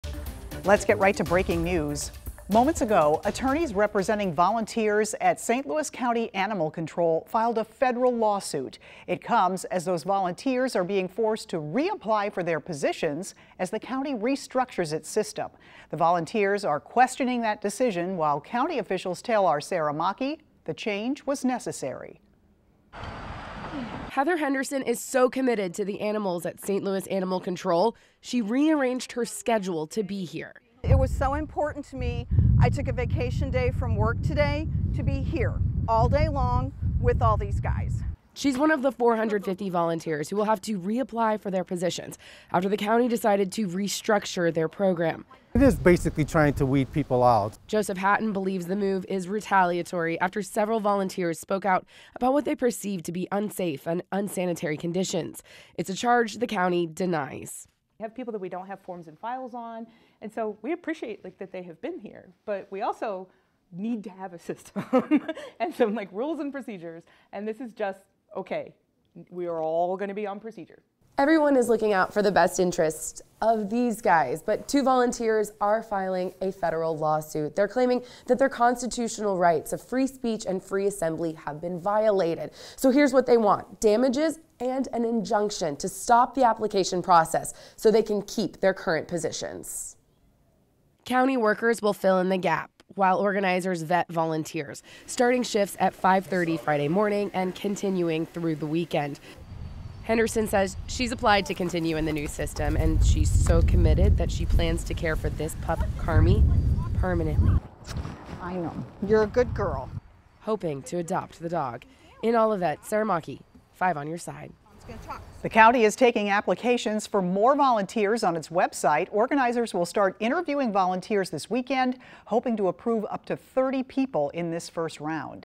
Federal lawsuit filed by St. Louis County Animal Shelter volunteers forced to reapply
Volunteers will have to vacate their posts and re-apply for their positions.